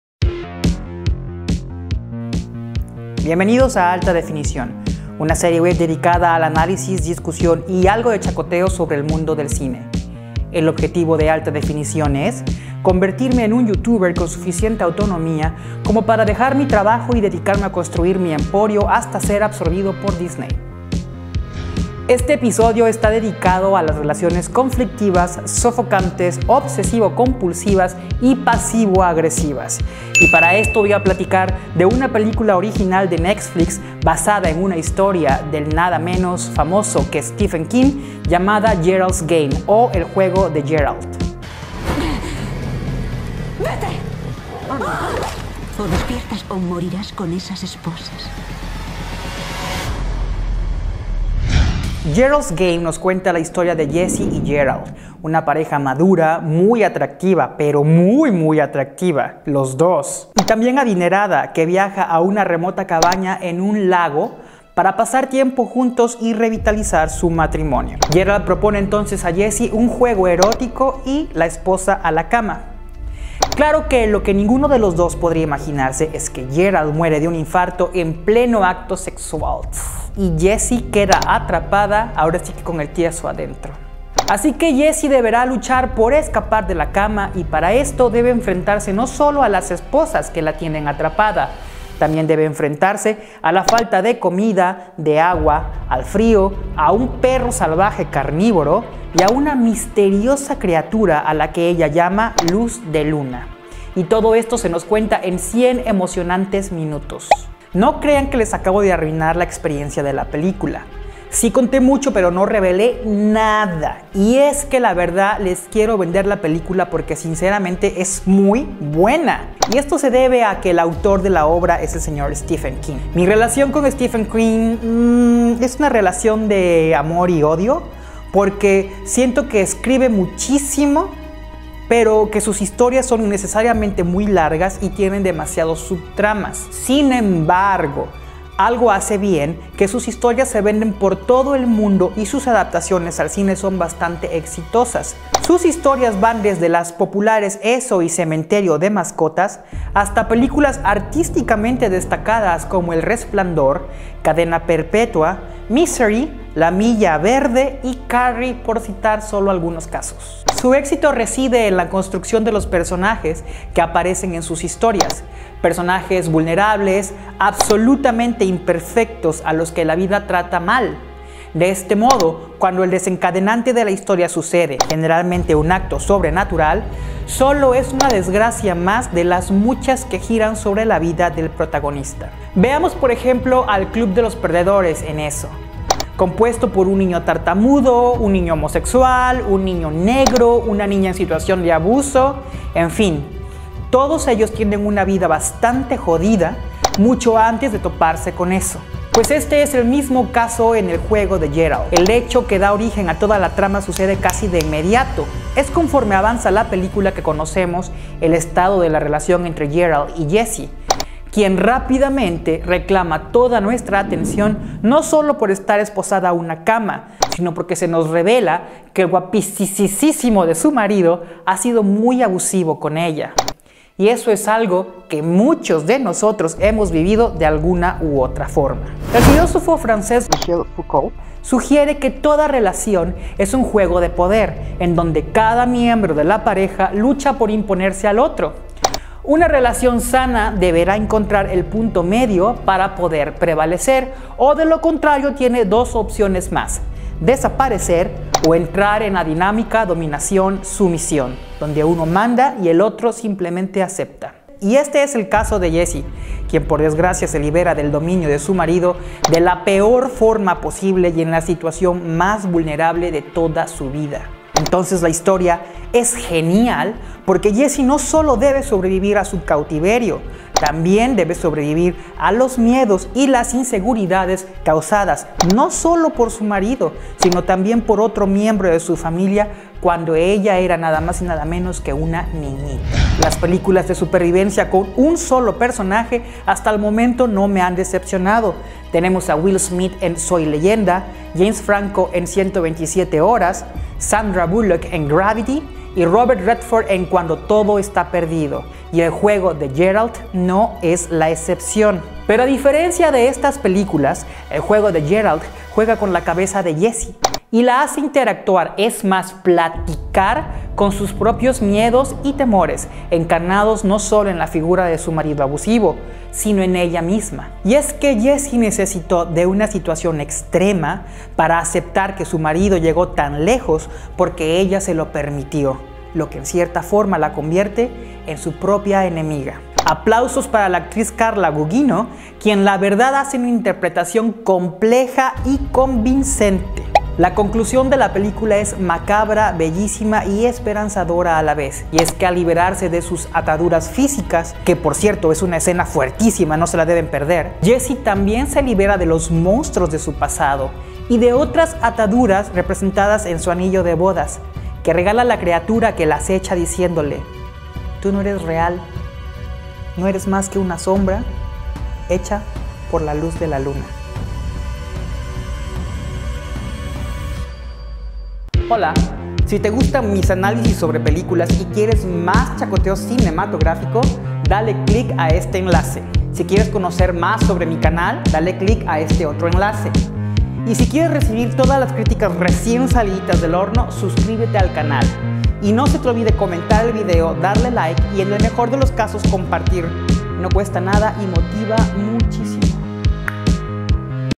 El Juego de Gerald (Gerald's Game) - Reseña de la película| Alta Definición
Pues la verdad que al pasar de los años la pasión entre una pareja se desgasta y si la pareja accede, los juegos sexuales pueden revitalizar la relación. Pero esposarse a la cama puede resultar mucho más peligroso de lo que se imaginan. Si no me creen vean el Juego de Gerald